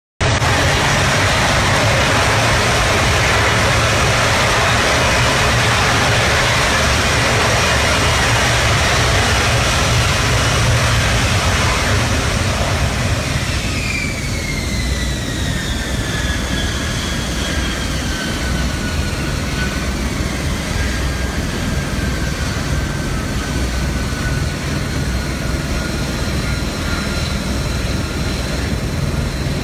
LHA America, embarked F-35B trials, part VI
USS America (LHA 6), United States Navy.
F-35B DT-III, United States Marine Corps.
Vid by commercial media, November 2016